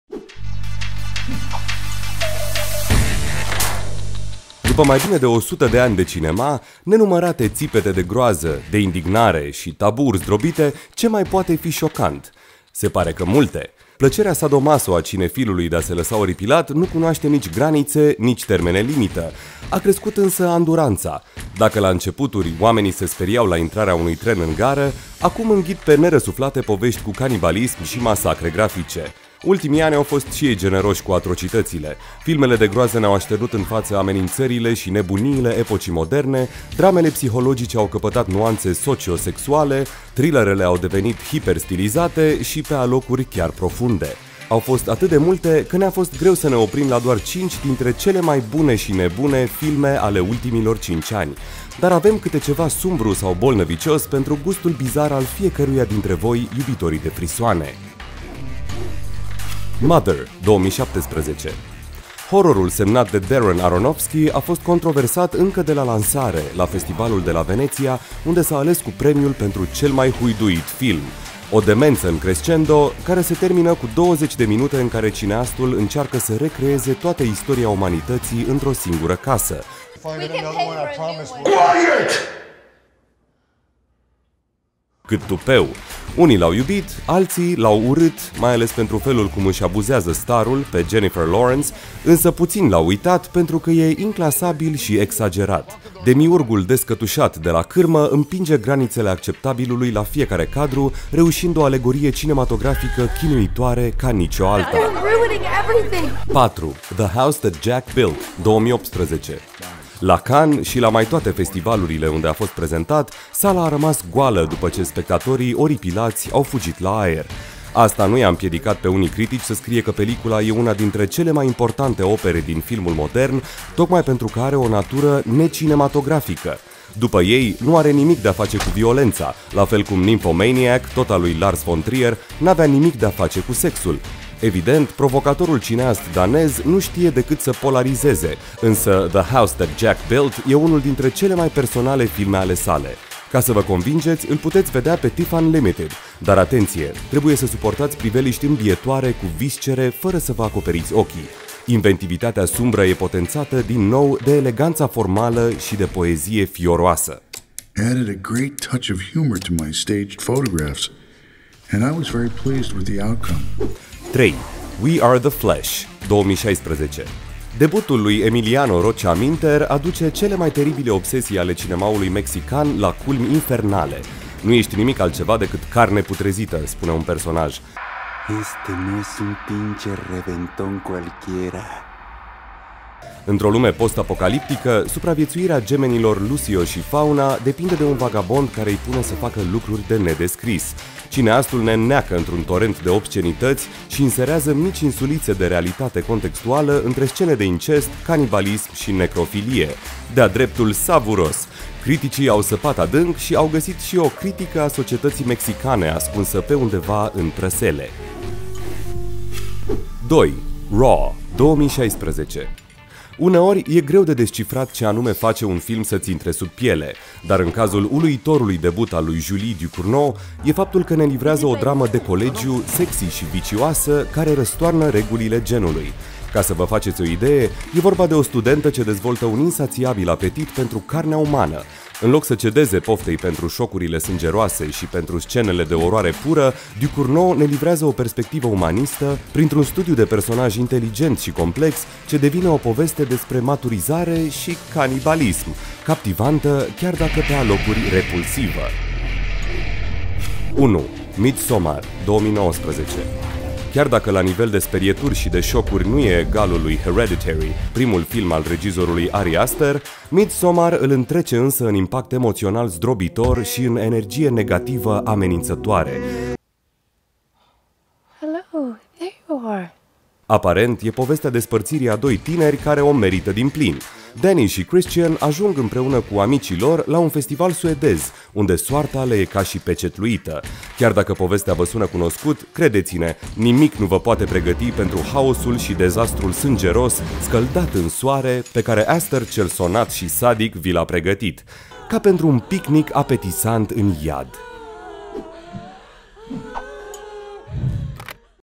High5 – Cele mai șocante filme din ultimii 5 ani
După mai bine de 100 de ani de cinema, nenumărate țipete de groază, de indignare și tabuuri zdrobite, ce mai poate fi șocant? Se pare că multe: plăcerea sado-maso a cinefilului de-a se lăsa oripilat nu cunoaște nici granițe, nici termene-limită. A crescut însă anduranța: dacă la începuturi, oamenii se speriau la intrarea unui tren în gară, acum înghit pe nerăsuflate povești cu canibalism și masacre grafice. Ultimii ani au fost și ei generoși cu atrocitățile: au fost atât de multe, că ne-a fost greu să ne oprim la doar 5 dintre cele mai bune și nebune filme ale ultimilor 5 ani. Dar avem câte ceva sumbru sau bolnăvicios pentru gustul bizar al fiecăruia dintre voi, iubitorii de frisoane.

Powered by AperiTIFF.